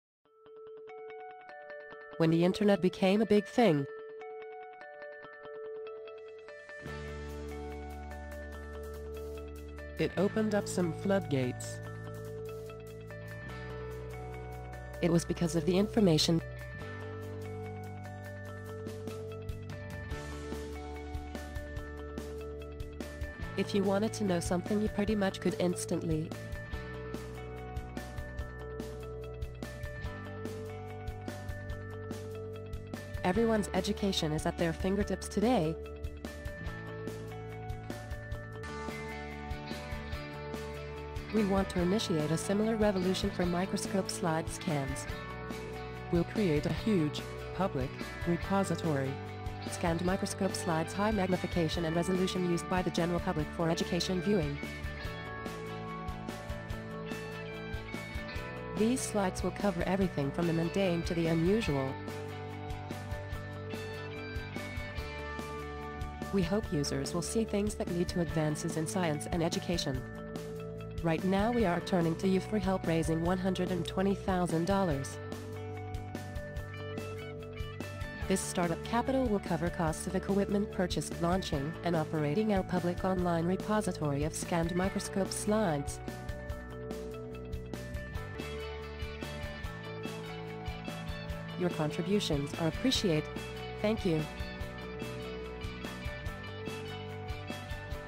The Microscope Slide Crowdfunding Project
When the internet became a big thing, it opened up a floodgate of information. This Crowdfunding Project looks to do the same for microscope slides.

Between $5 - $50!